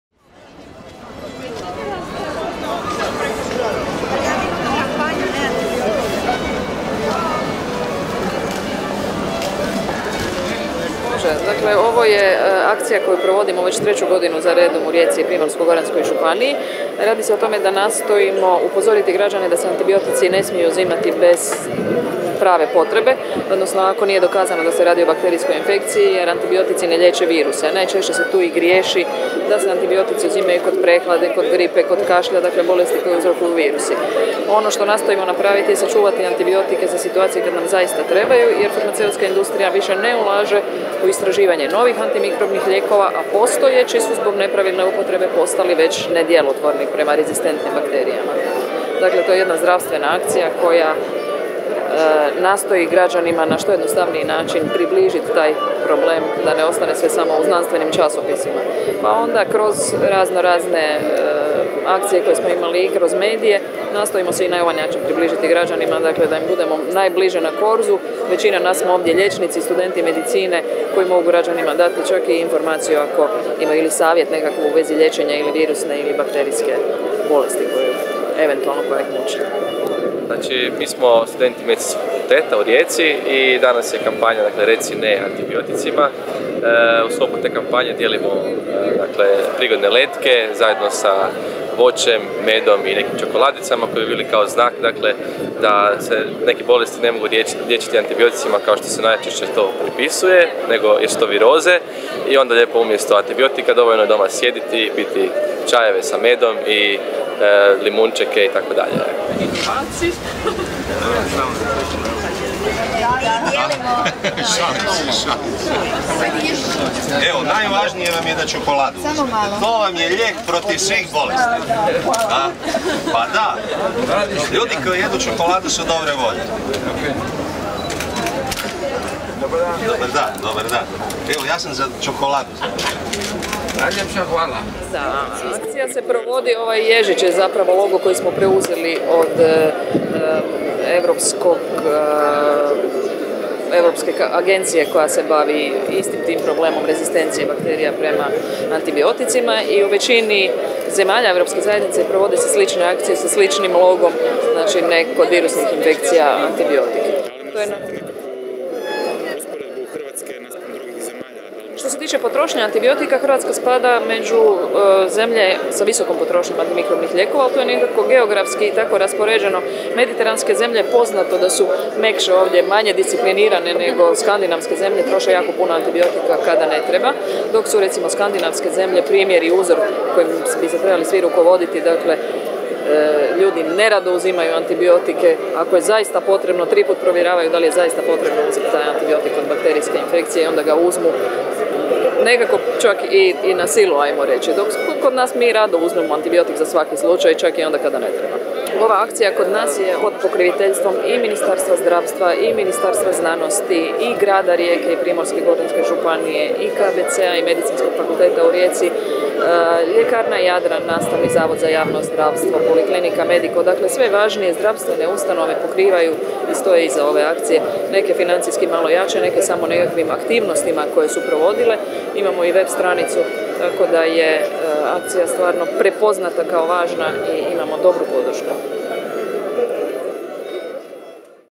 MojaRijeka.hr - Akcija JEKA (javna edukativna kampanja o ispravnoj uporabi antibiotika)
Gripa ili prehlada
Reci "NE" antibioticima

Svrha akcije je prenijeti građanima sljedeće poruke:
- antibiotici su lijekovi koji liječe bakterijske infekcije, ali ne i virusne;
 - obična prehlada, gripa, kašalj, hunjavica, većina upala gornjih dišnih puteva virusnog su porijekla i ne mogu se liječiti antibioticima;
- svakom uporabom antibiotika, bila ona opravdana ili ne, smanjuje se djelotvornost antobiotika zbog razvoja otpornosti bakterija;
- antibiotici se smiju uzeti samo uz liječničku preporuku -- ne uzimajte antibiotike „na svoju ruku" - čuvajmo antibiotike za situacije kad su stvarno potrebni.